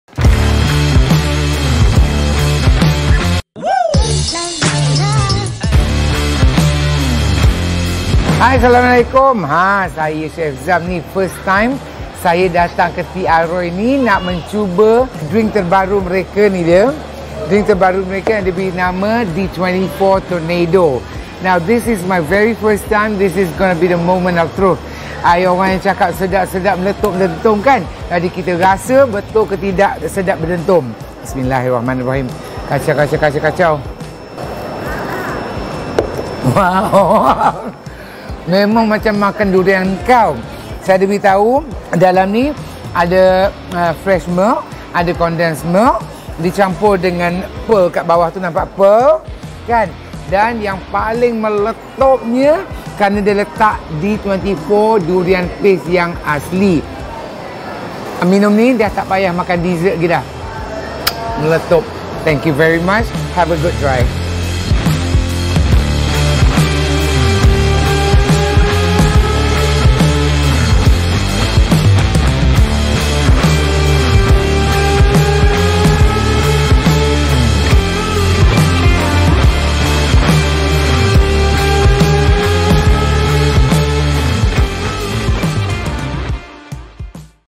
Air Durian D-24 Tornado Paling Meletop!
Haa tu dia Tea Aroi, lain macam sekarang sebab dah ada air durian - D24 Tornado, memang rasa pure siap dengan pearl lagi!

Bak kat chef Zam, minum D24 Tornado ni tak payah makan pencuci mulut dah sebab bahan yang mereka gunakan memang berkualiti dan asli.

Ini memang minuman wajib cuba kalau korang peminat tegar durian. Cepat terjah cawangan mereka kat di LG2 Sunway Pyramid (berdekatan The Manhattan Fish Market)!